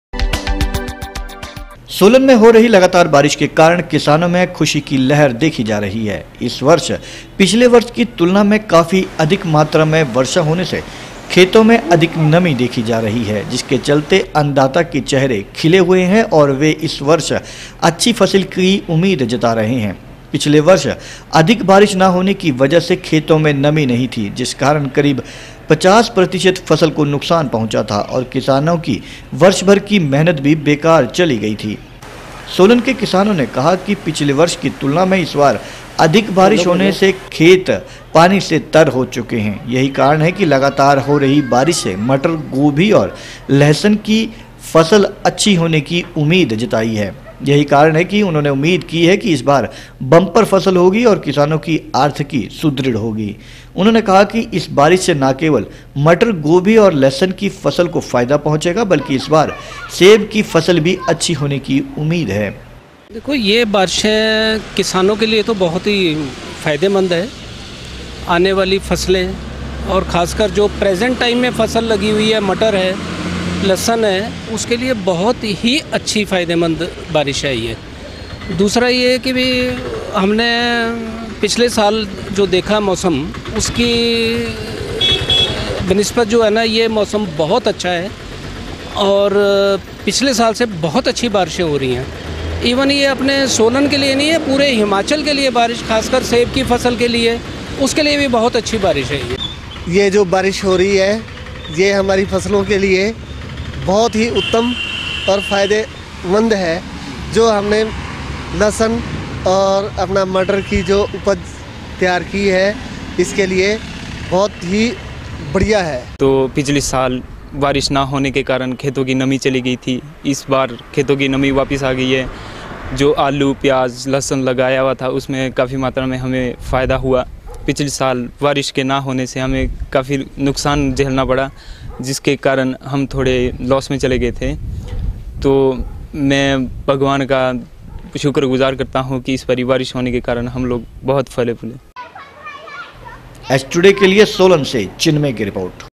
सोलन में हो रही लगातार बारिश के कारण किसानों में ख़ुशी की लहर
सोलन में हो रही लगातार बारिश के कारण किसानों में ख़ुशी की लहर देखी जार ही है इस वर्ष पिछले वर्ष की तुलना में काफी अधिक मात्रा में वर्षा होने से खेतों में अधिक नमी देखी जा रही है जिसके चलते अन्नदाता के चेहरे खिले हुए है और वह इस वर्ष अच्छी फसल की उम्मीद कर रहे है पिछले वर्ष अधिक बारिश न होने की वजह से खेतों में नमी नहीं थी जिस कारण करीबन 50 प्रतिशत फसल को नुक्सान पहुंचा था और किसानों की वर्षभर की मेहनत बेकार चली गई थी